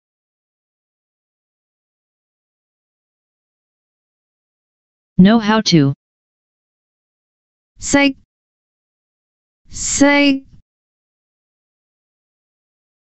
"KNOW HOW TO" in Cantonese (識) - Flashcard
"KNOW HOW TO" in Cantonese (識) - Flashcard. Additional information about the word: know,recognize.